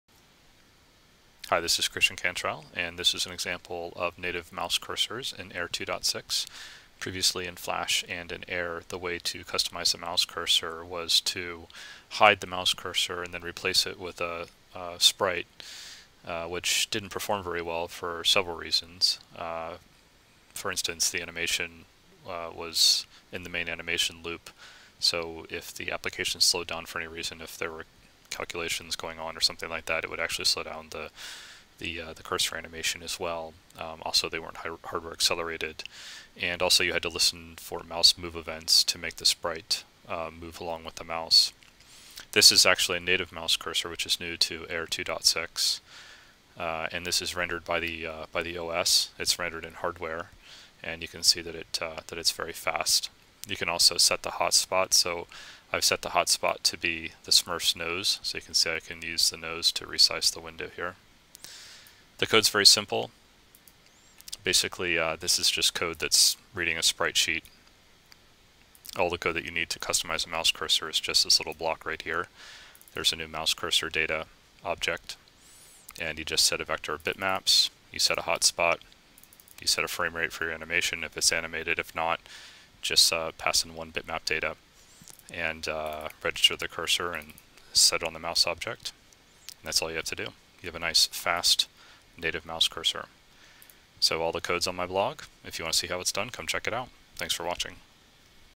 Demo of Native Mouse Cursors in Adobe AIR
Here's a quick demo of an animated native mouse cursor in Adobe AIR.